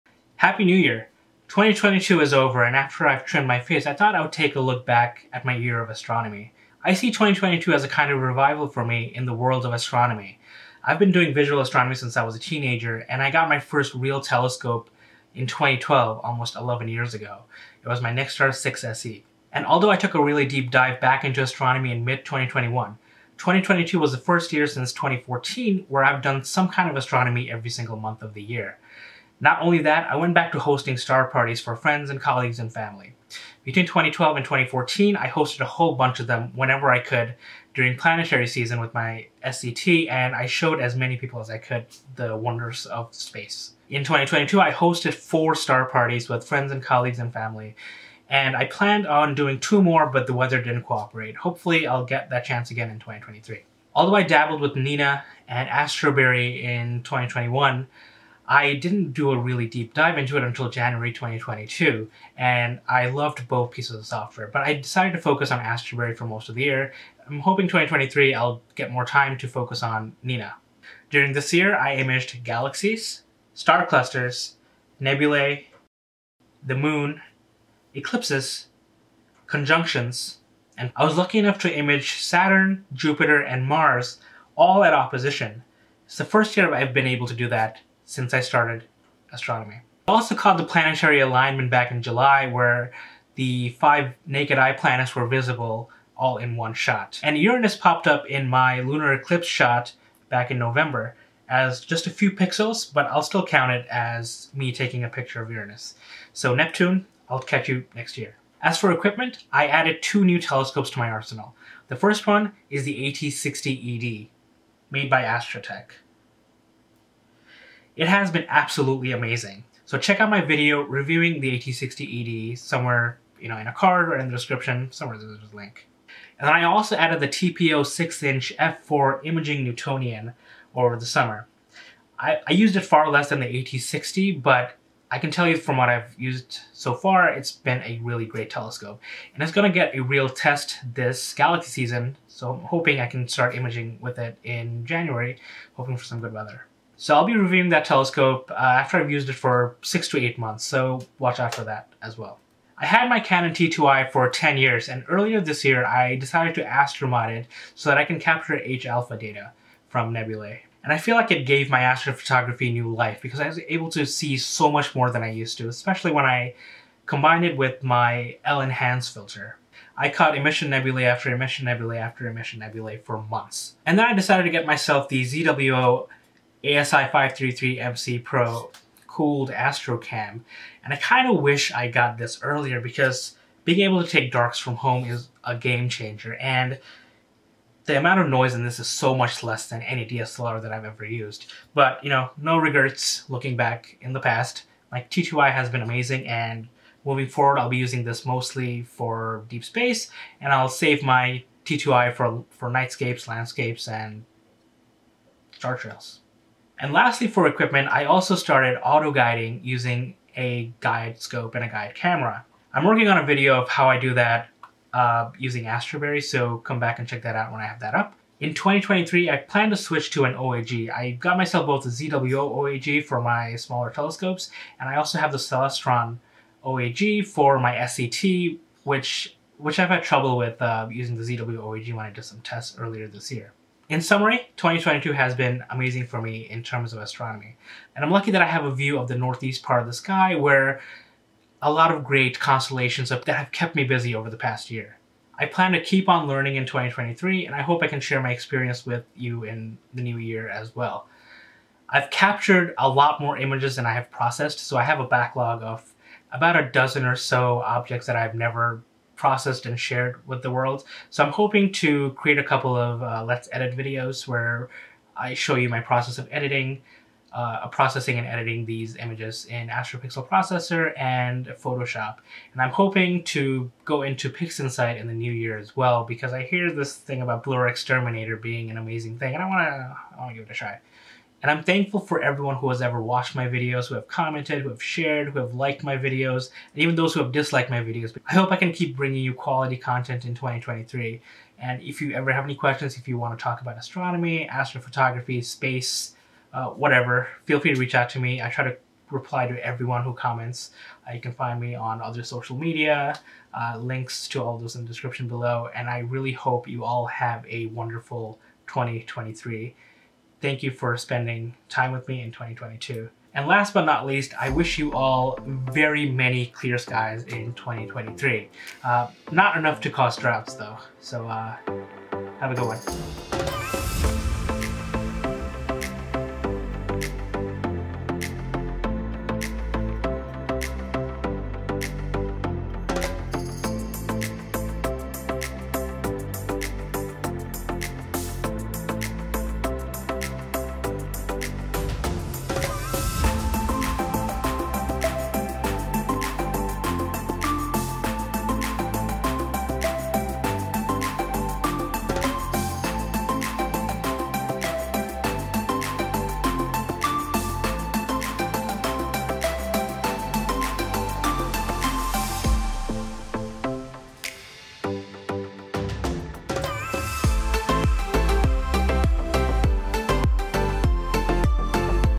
My Astronomy Year in Review and Looking ahead to the new year
2022 is over and I had an amazing year of Astronomy. In this video, I go over just some parts of my year and the amazing objects I was able to image. Everything from Mars, Jupiter, and Saturn at opposition to countless galaxies and star clusters, unimaginable nebulae, and of course, the Earth's Rotation while we focus on the Milky Way Galaxy.

I hope 2023 is wonderful for all of you. Thank you for letting me be a part of your life in 2022!

Chapters:
0:00 Intro
1:50 My New Gear of 2022
3:53 Summary
5:20 Final look at some of my Astrophotography

Have questions? Ask below! Be sure to check out my Astrophotography